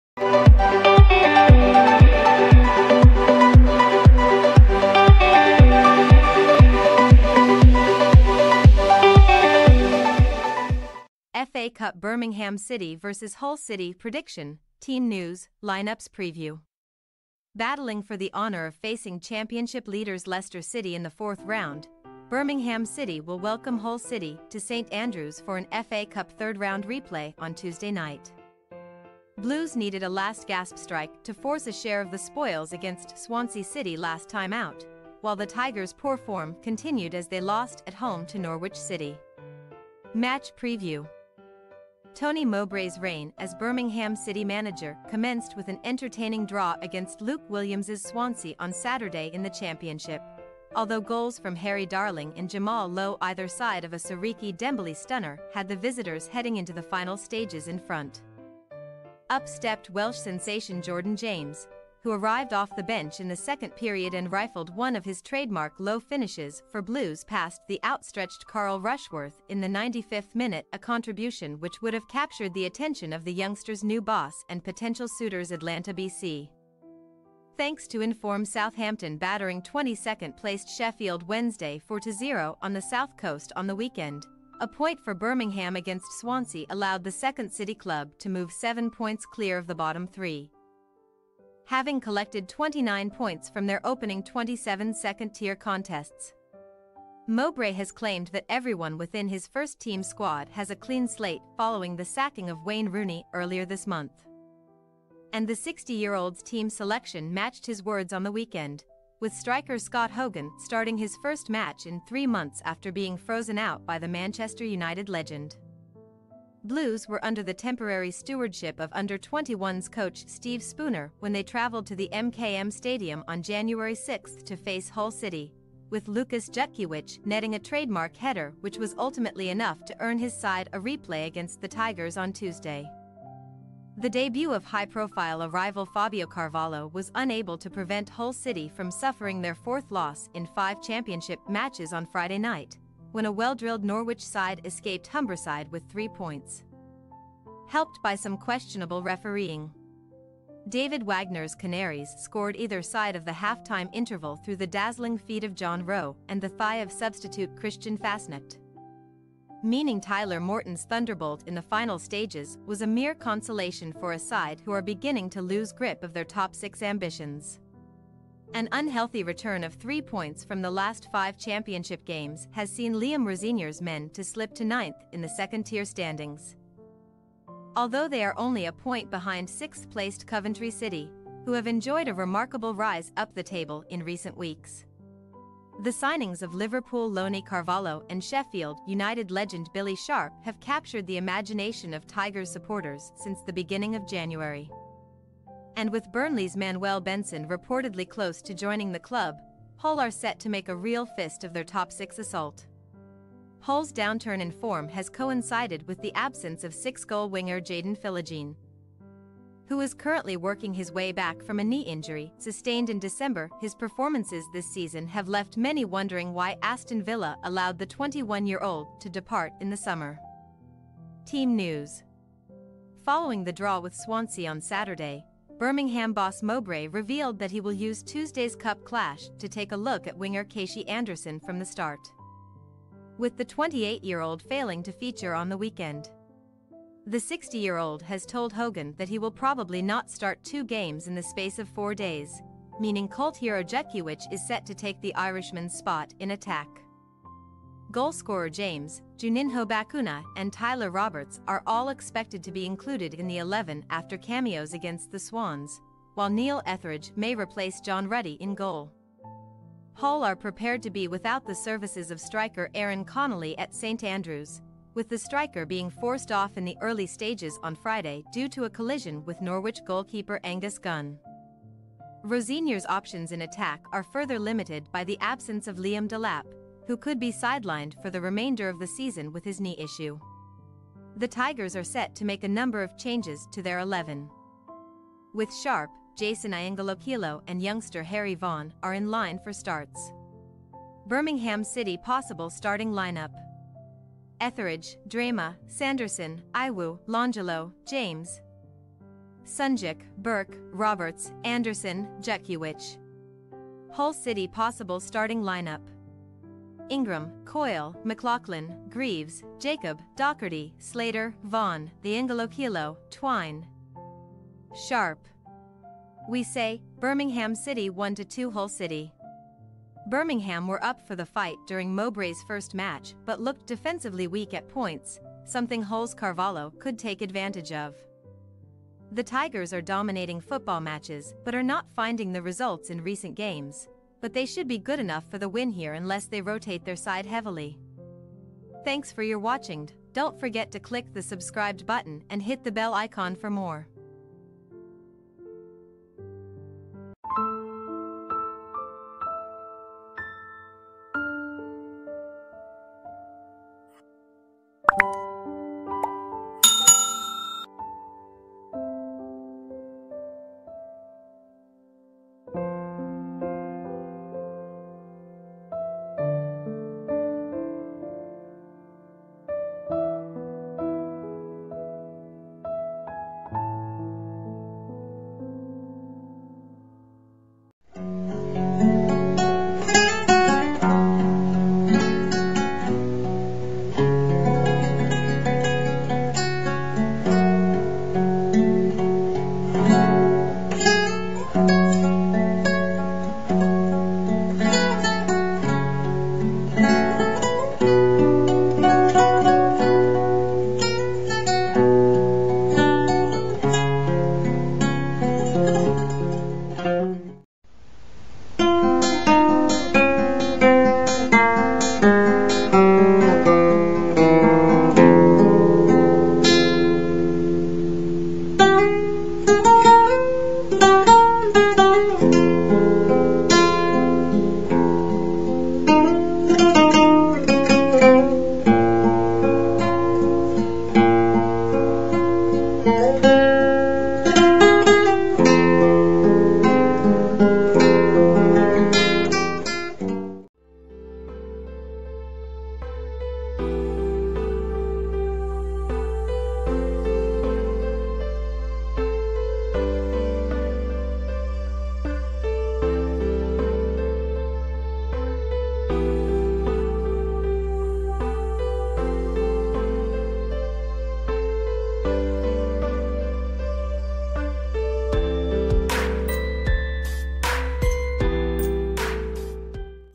FA Cup | Birmingham City vs. Hull City - prediction, team news, lineups | Preview
Battling for the honour of facing Championship leaders Leicester City in the fourth round, Birmingham City will welcome Hull City to St Andrew's for an FA Cup third-round replay on Tuesday night.

00:00 Introduction
00:11 Contents